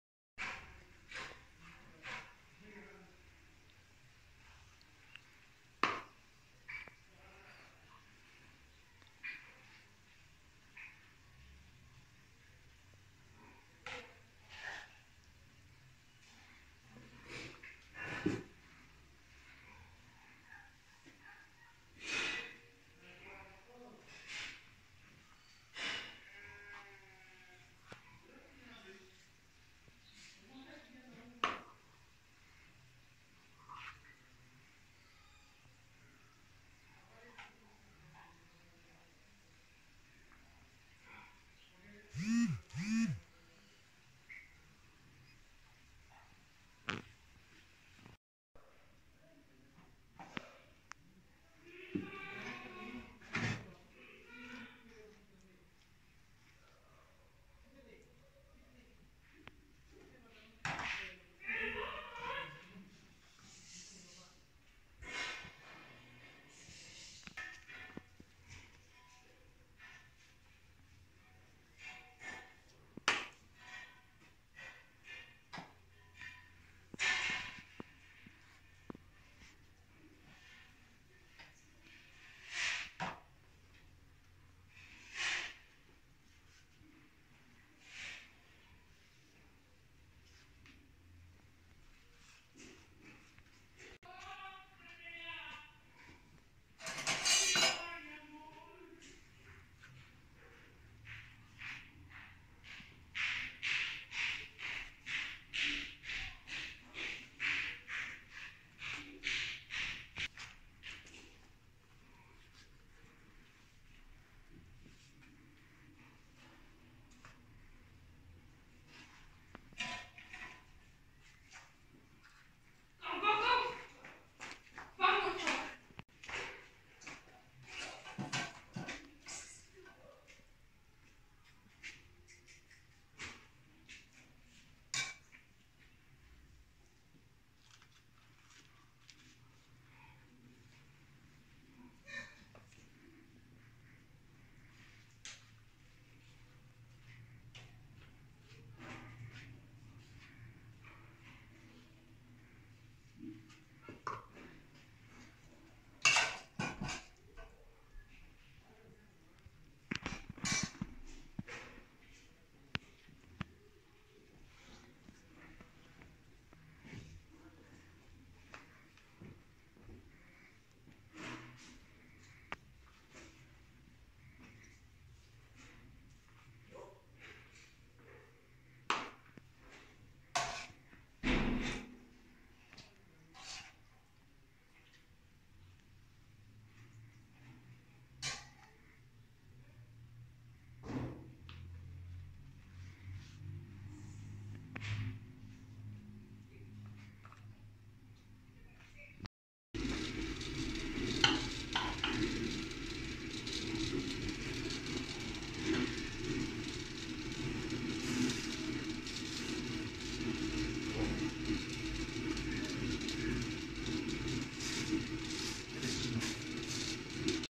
Mi mami preparando unos ricos tamalitos de mole rajas y dulce 😋Santa María Xonacatepec Puebla
Gracias mami por prepararnos unos ricos tamalitos de mole rayas y dulce aquí en santa maría Xonacatepec Puebla